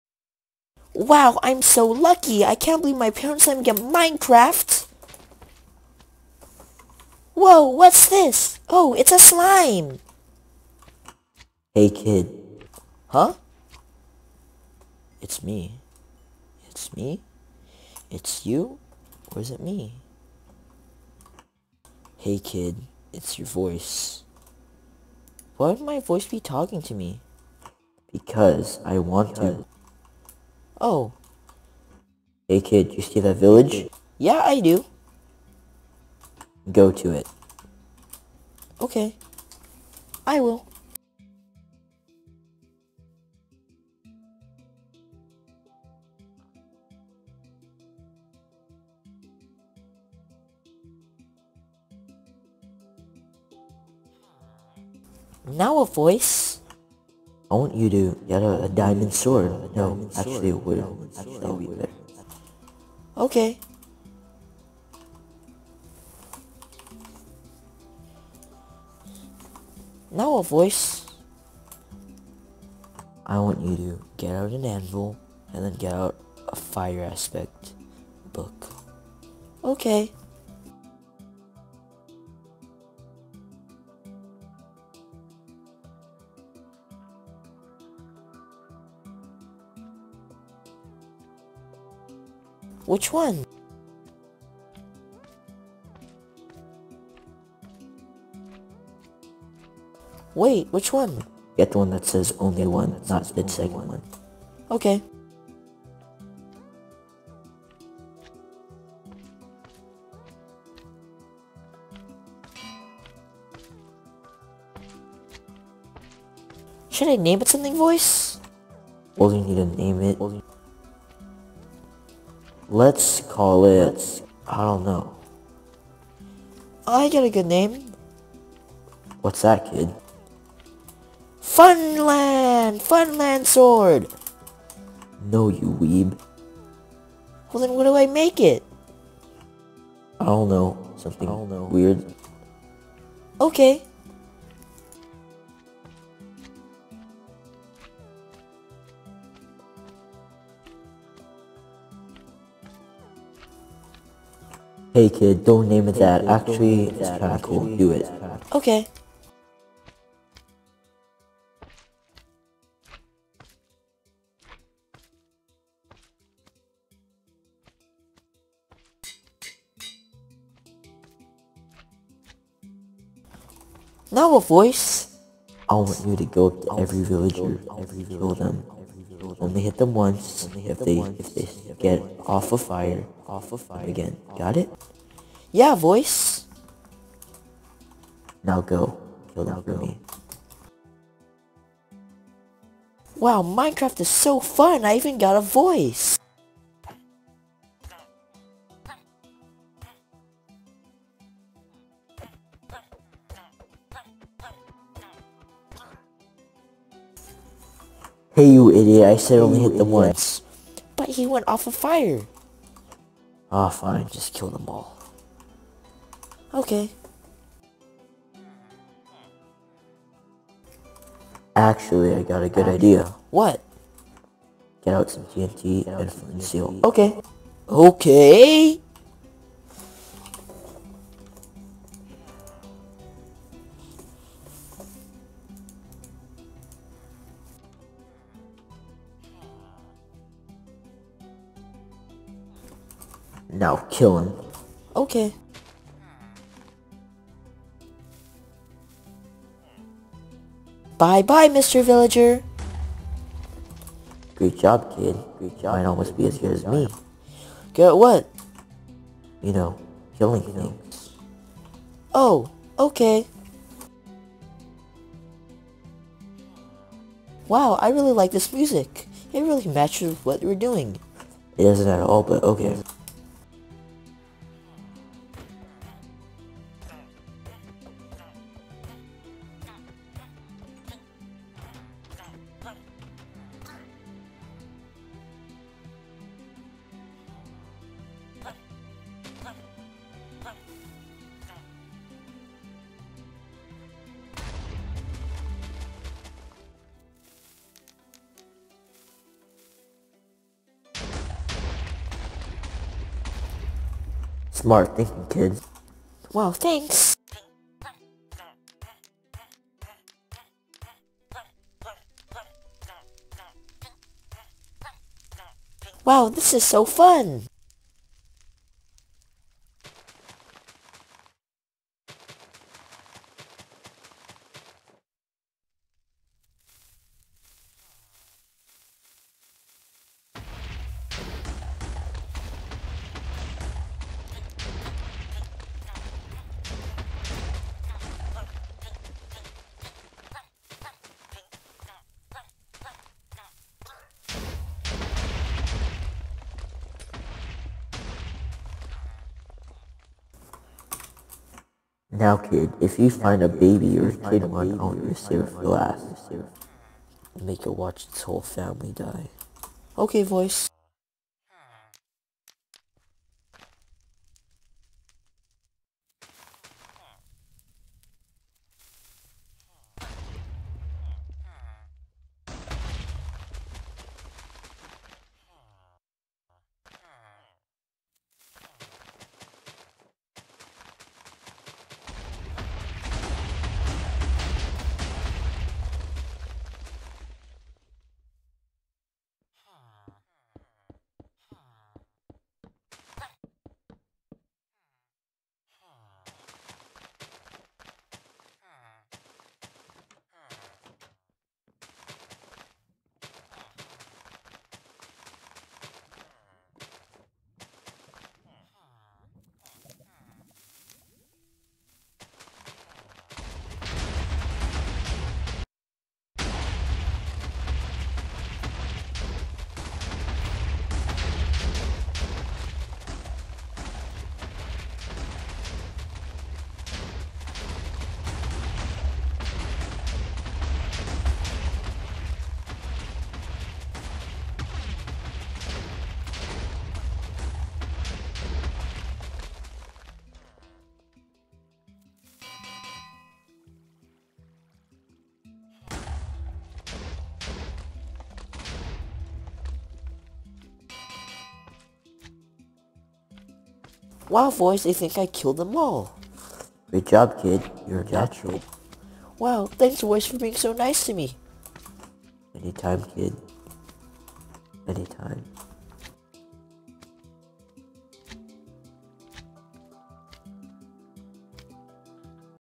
My Voice EP 1
A series which follows a child who has a crazy voice in his head trying to take over the child. DAB DAB DAB DAB FOR THE LOLZ